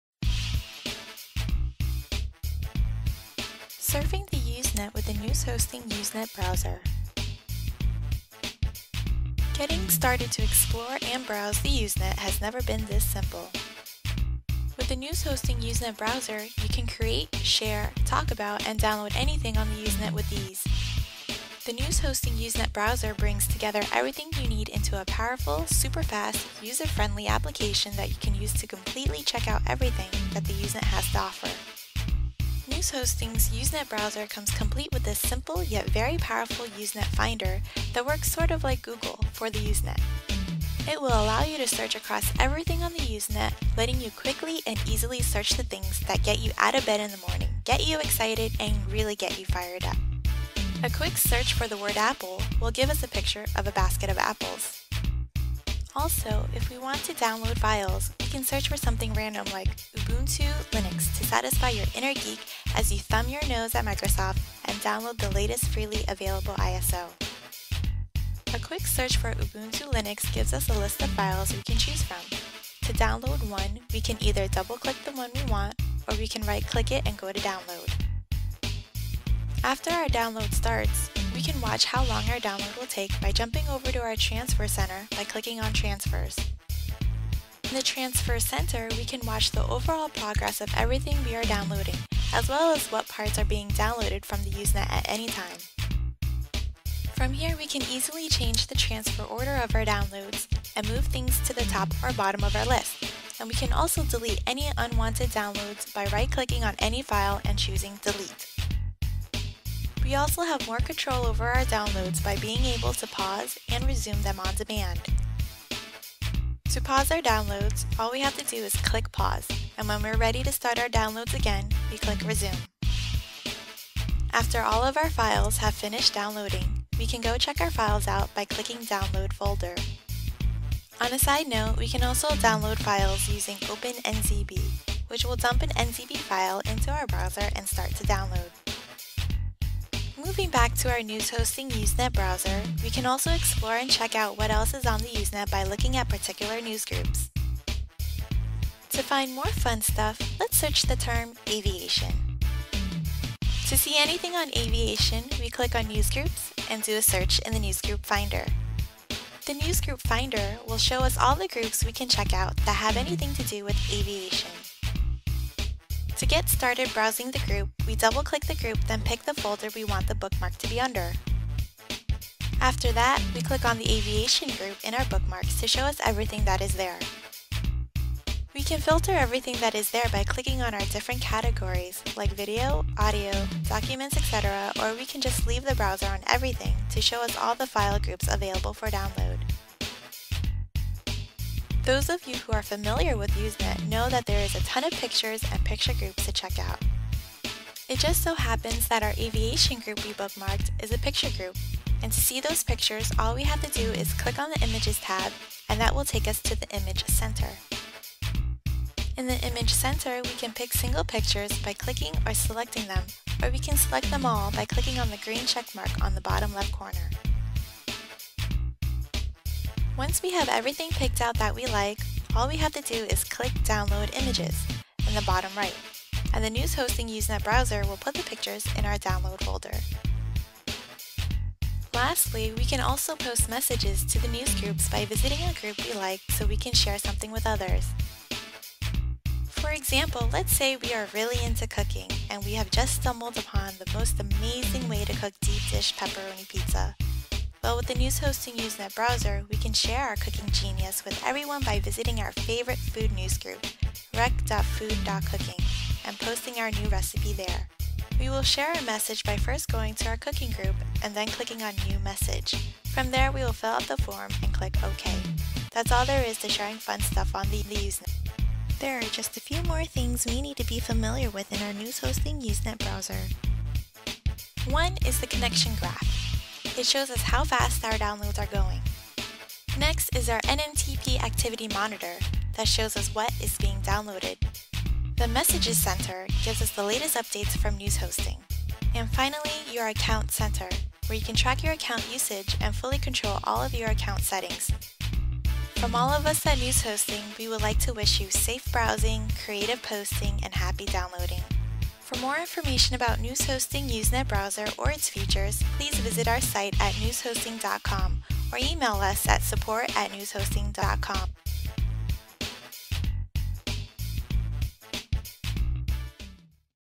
Newshosting Usenet Browser - Surfing the Usenet....
Newshosting Usenet Browser - In this video we show how quick, easy and fun surfing and downloading from the Usenet can be with the Newshosting Usenet Browser - check it out, you will be downloading and talking with others around the world in no time :)